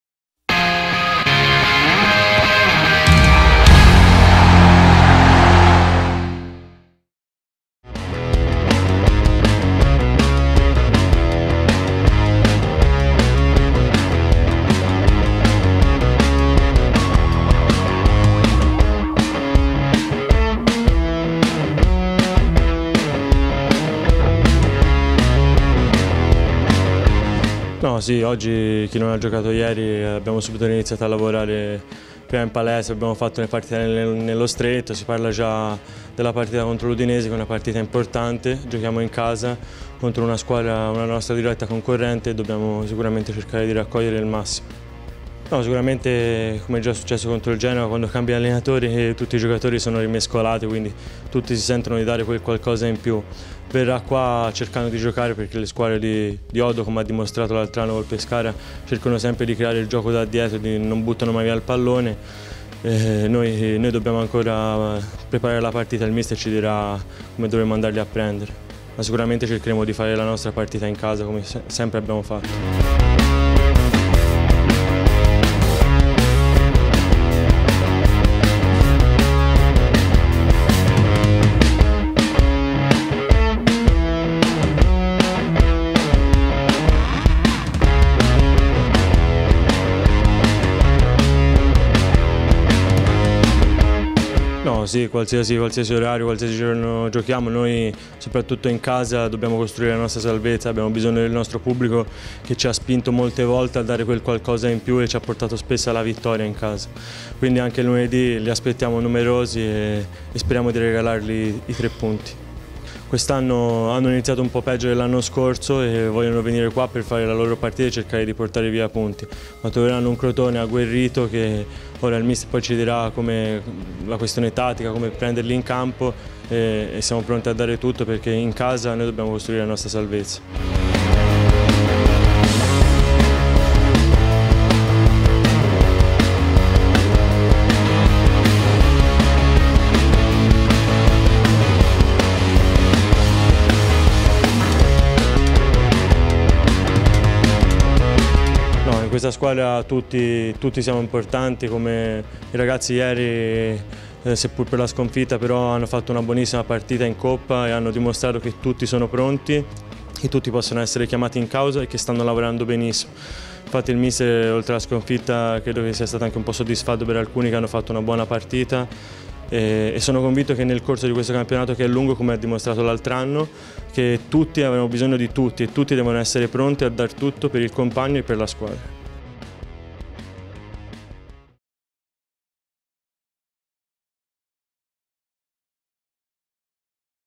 Prima seduta in vista dell’Udinese, la carica di Ceccherini
Tornati nella notte da Genova, gli squali hanno usufruito di qualche ora di riposo per poi ritrovarsi nel pomeriggio di oggi al centro sportivo per sostenere il primo allenamento in vista della gara di campionato contro l’Udinese, in programma lunedì alle 19 allo Scida.
Il giorno dopo la gara, la seduta è stata dedicata al recupero per chi è stato schierato titolare al Marassi, forza in palestra e partite a pressione per il resto del gruppo.
Kragl e Izco che proseguono nel loro percorso di recupero.
Domani di nuovo in campo, al mattino, sempre al centro sportivo.
A fine allenamento è intervenuto ai microfoni di Fctv Federico Ceccherini: “Contro l’Udinese sarà difficile, hanno cambiato da poco allenatore, giocheremo contro una nostra diretta avversaria e lo faremo in casa, dove dobbiamo costruire la nostra salvezza e avremo dalla nostra la spinta del pubblico. Tutti siamo importanti, abbiamo bisogno uno dell’altro, siamo un grande gruppo e lo abbiamo dimostrato anche ieri in Coppa con una buona prestazione”.